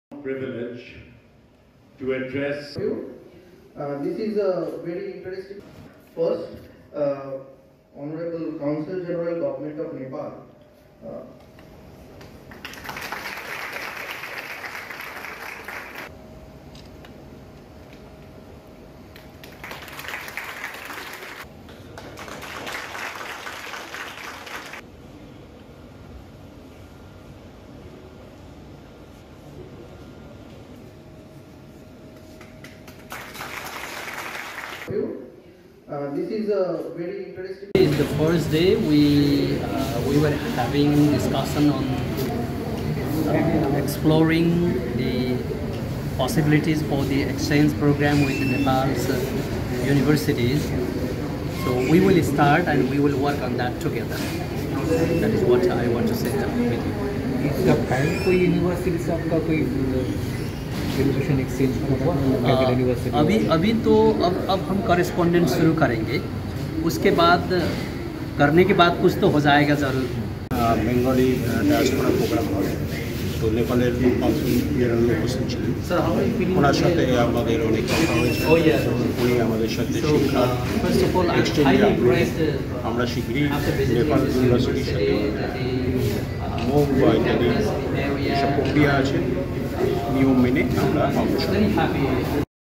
নেপালের শিক্ষা-সংস্কৃতি আদানপ্রদানের উদ্যোগ
কলকাতা এবং কল্যাণীর মত বিশ্ববিদ্যালয়গুলির সঙ্গে শিক্ষা-সংস্কৃতির আদানপ্রদানে উদ্যোগী নেপালের বিশ্ববিদ্যালয়। কল্যাণী বিশ্ববিদ্যালয়ের সেন্টার ফর স্টাডিজ অন বেঙ্গলি ডায়াসপোরার উদ্যোগে আয়োজিত সম্মেলন।